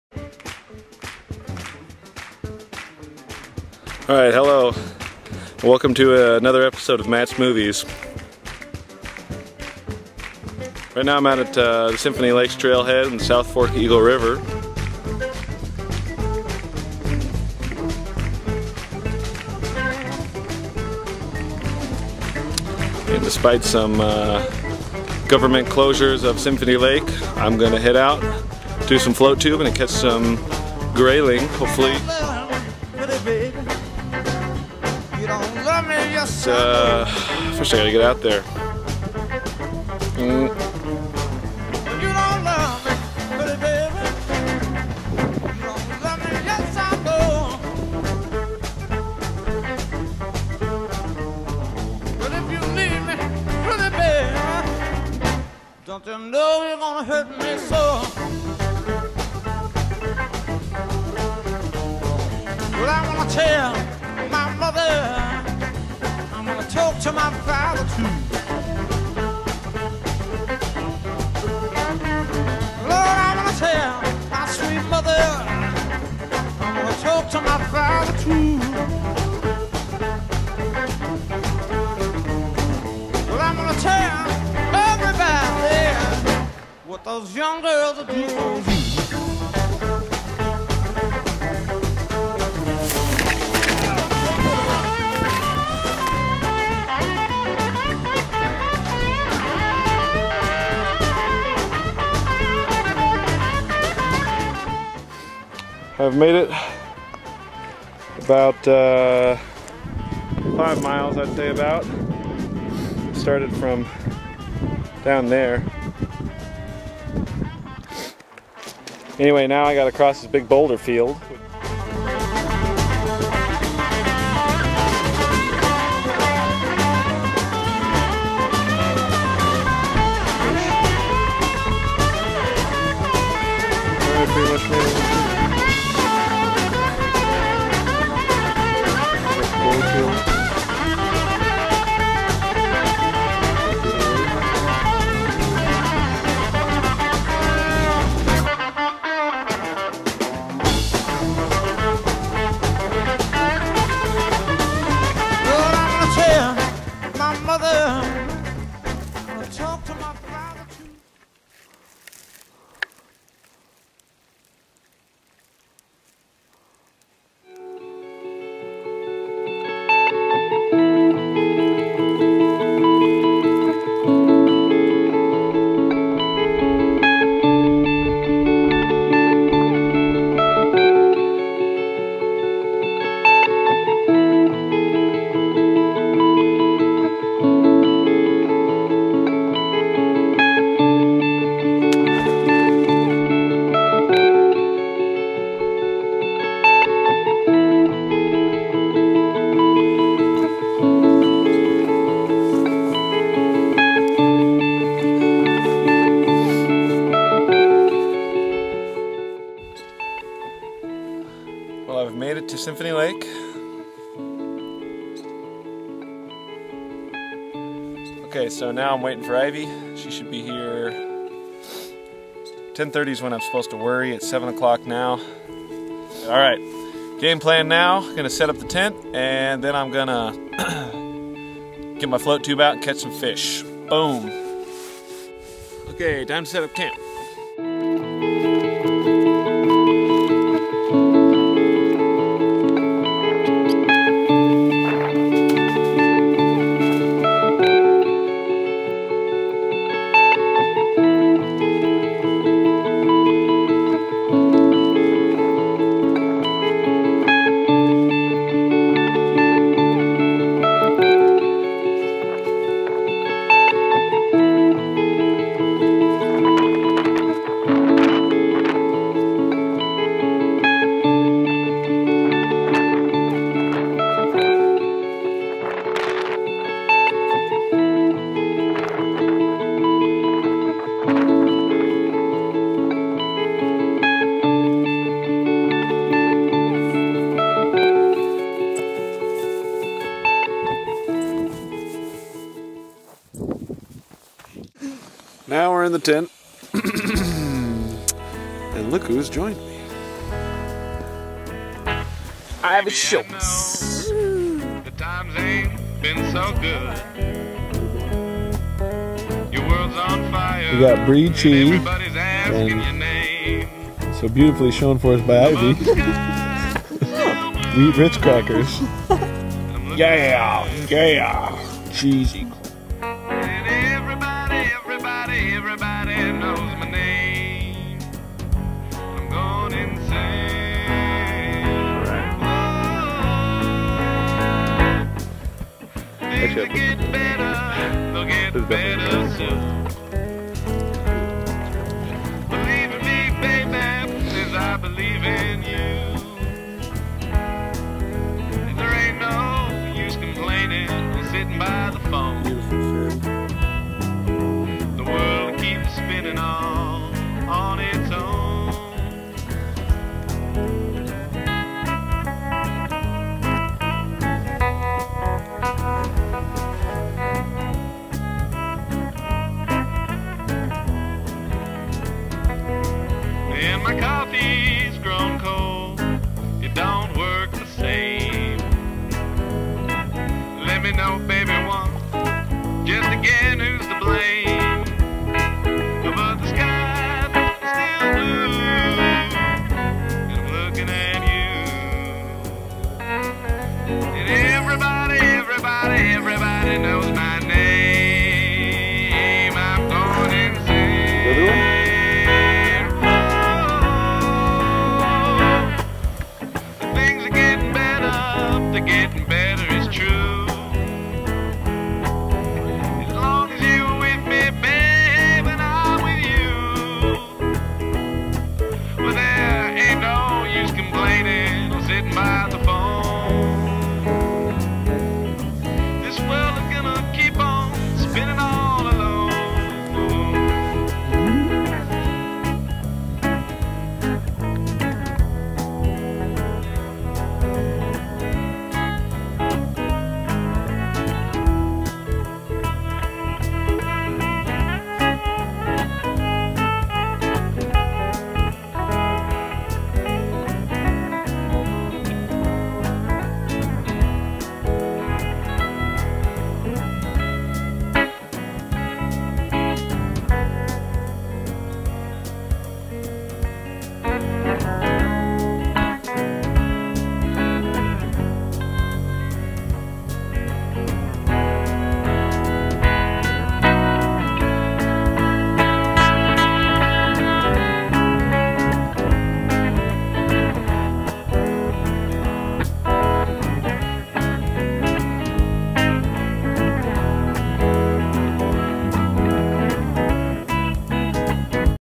Matt's Movies: Remote Fly Fishing in Symphony Lake, Alaska
MATT"S MOVIES.  Hiking, Backpacking, and Fishing at Symphony Lake, a remote high-alpine lake outside of Eagle river alaska.